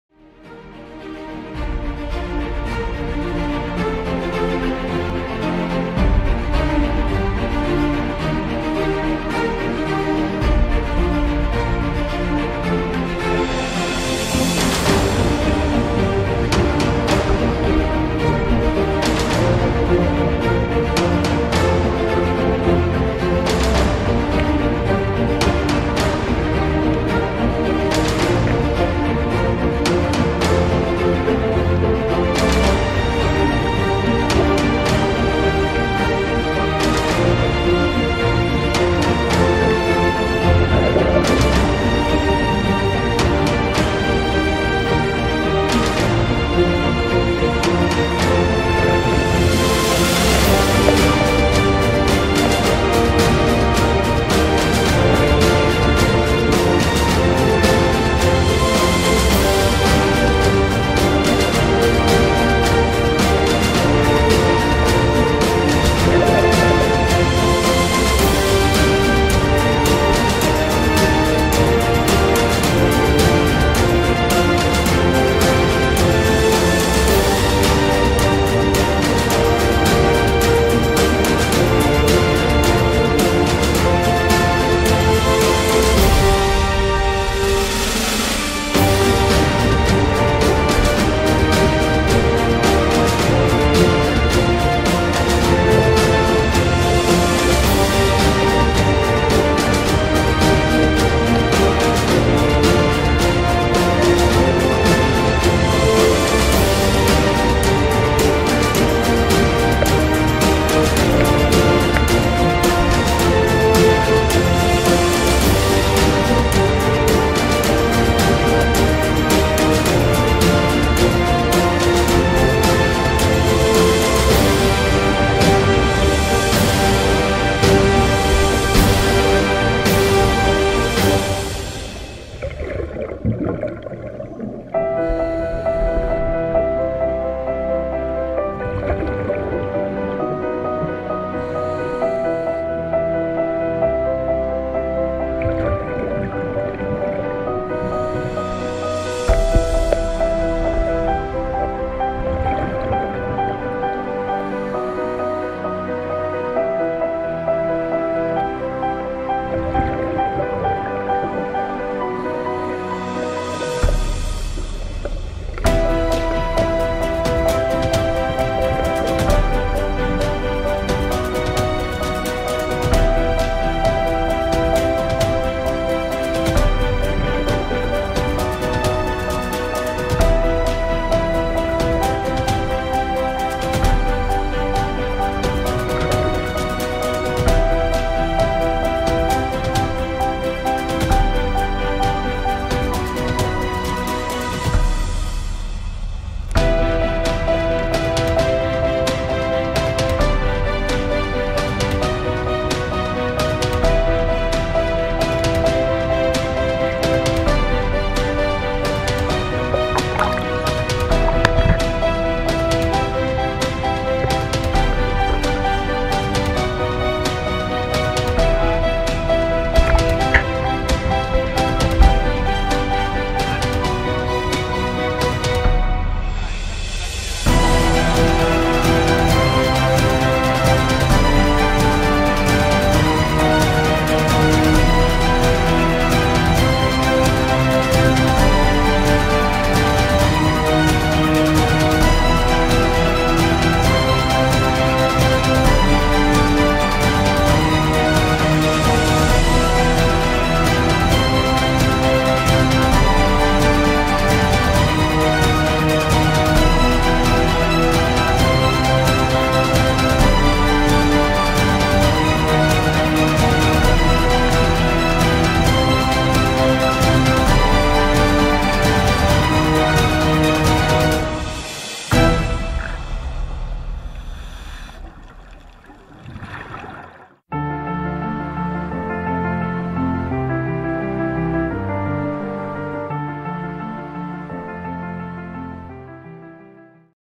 IMMERSIONE NELLA GROTTA ROVERSI
immersione del 17/05/2020
una tra le grotte piu' belle del salento...
Santa Caterina di Nardo' ( LE )